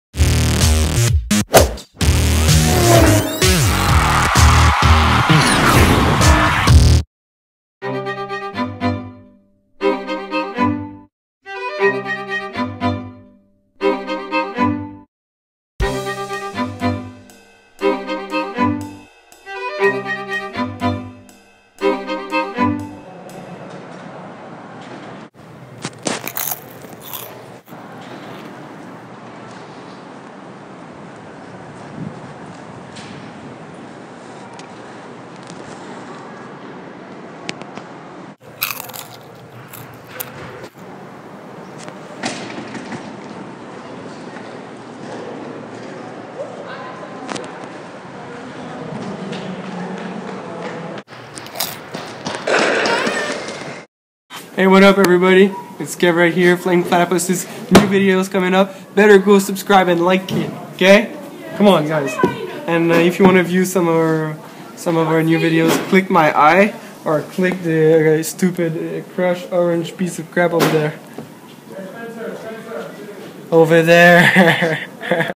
Kill James Part 2
James has returned from the dead, this time a tag team will attempt to defeat him, but will it put him to rest once and for all? Watch and Find out

Credits: James (stunt guy) Marek (Camera Guy) Chris and Kevin (ending)

iMovie app used with garage band for sound effects and music